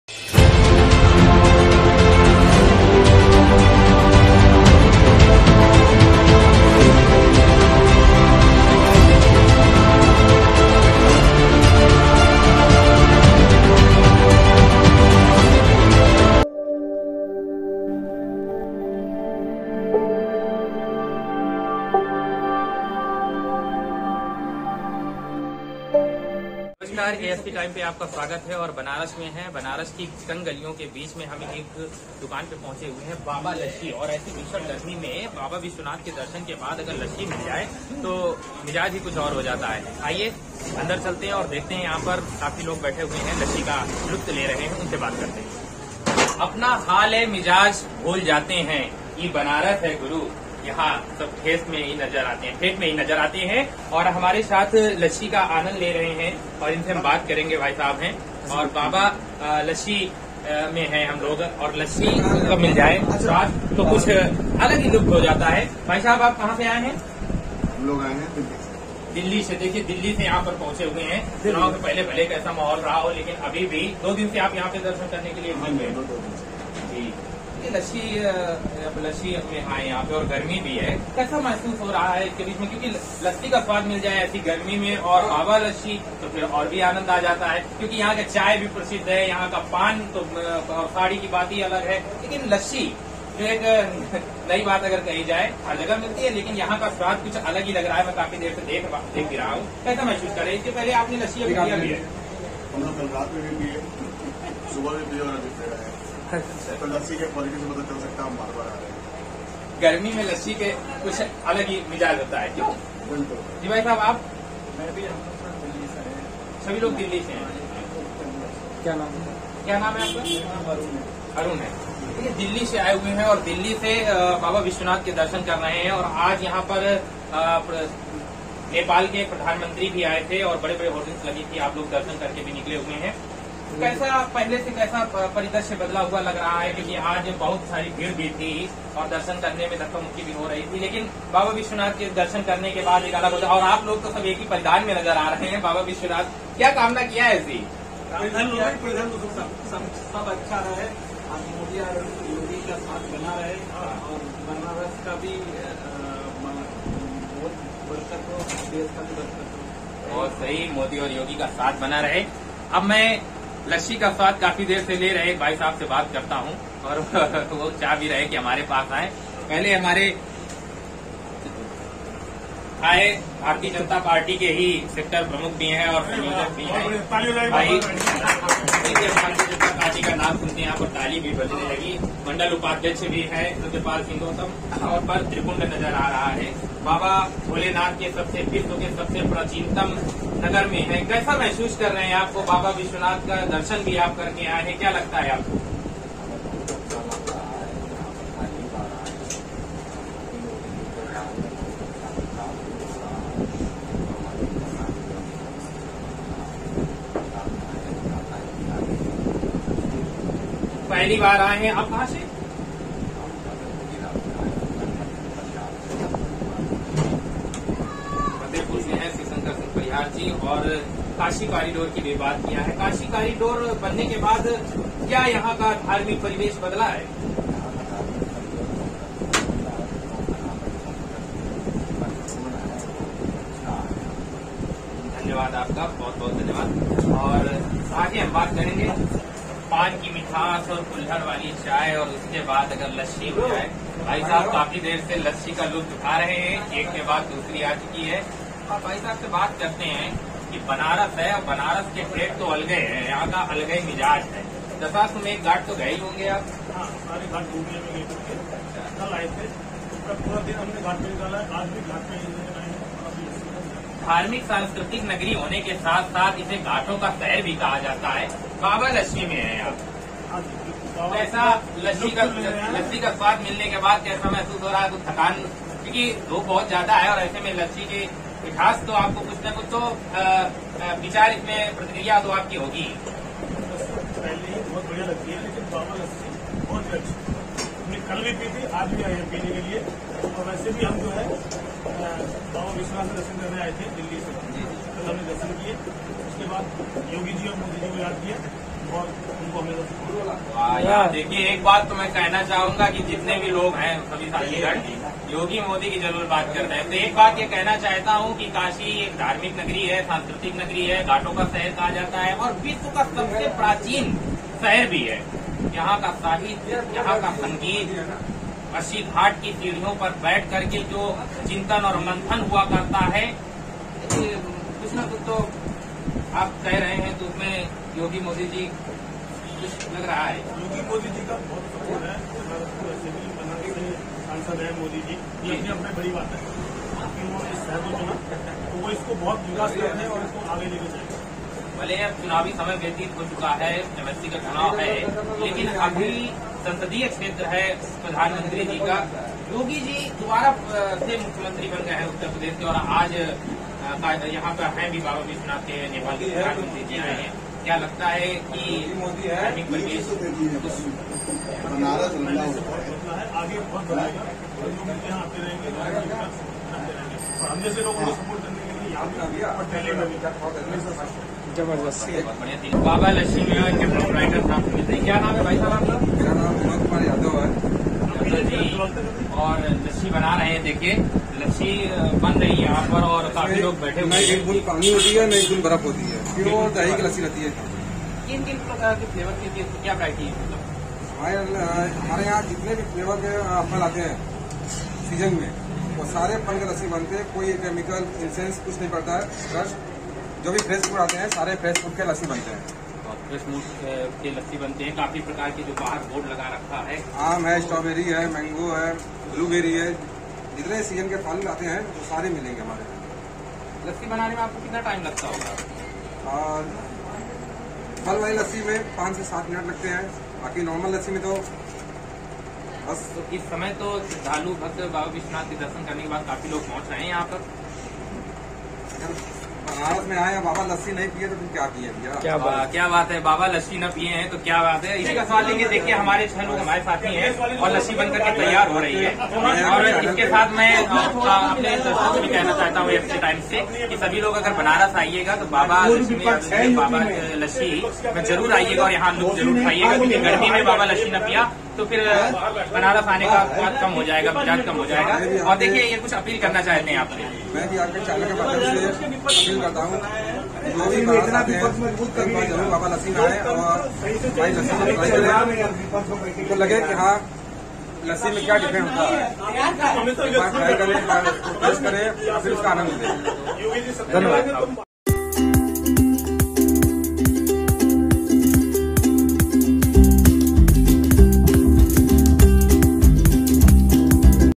आपने आर्गेनिक फलों की बनी लस्सी पी है क्या, अगर नही तो आपको मिलाते है बाबा लस्सी से..... || Banaras
InstagramLink:-

Effect credit-
MTC Tutorials